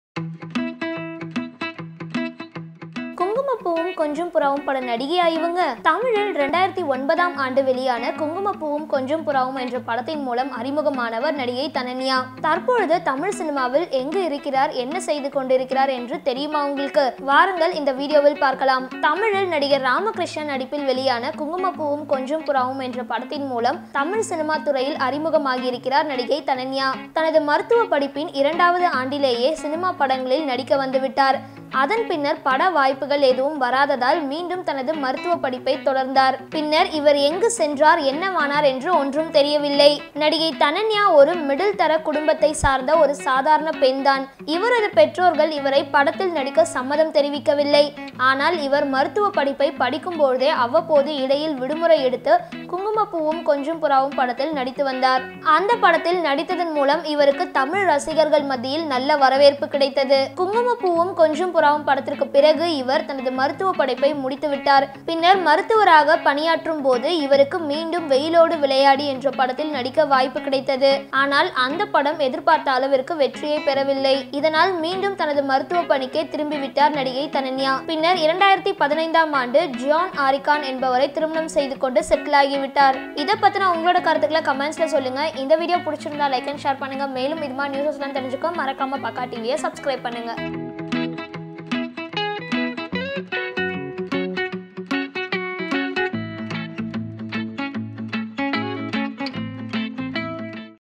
குங்கும பூவும் கொஞ்சும் புறாவும் பட நடிகை இன்றைய நிலை?
Thananya is an Indian film actress who entered the film industry through her debut movie Kungama Poovum Konjum Puravum. She was pursuing her degree in medicine and was third year student at that time and so after this film, took a short break to complete her course.

Her father’s name is Dr. Sudhakar and her mother’s name is Dr. Vijayalakshmi. She belongs to an affluent family and her parents are also doctors and they did not favor her performance in the film industry especially in the middle of her studies. However, she took leave for a month and completed the film. She has one brother. She was originally interested in modeling and getting an offer in the film was a pleasant surprise for her.

When she was working as a house surgeon, she got the offer to act in the film Veyilodu Vilayadu. She reentered through the film Veyilodu Vilayadu which was centered on the games of Kabaddi, cricket and football. This film was successful and got her good name. This film was about a young person interested in volleyball which took him to the pinnacle of glory. The real life volleyball champion Magesh played the lead role in this film.